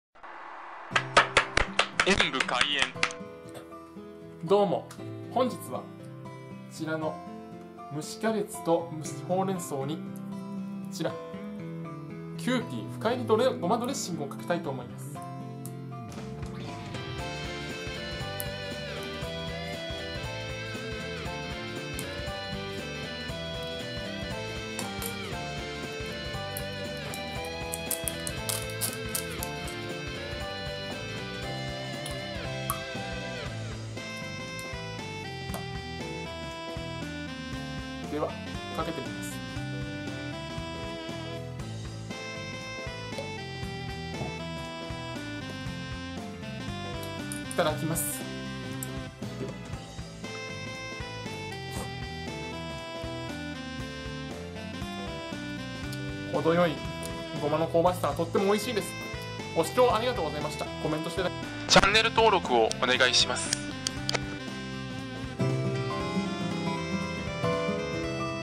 キューピー 胡麻ドレッシングを使ってみた
キューピー　胡麻ドレッシングを使ってみました。
ドレッシングでも香ばしいですね～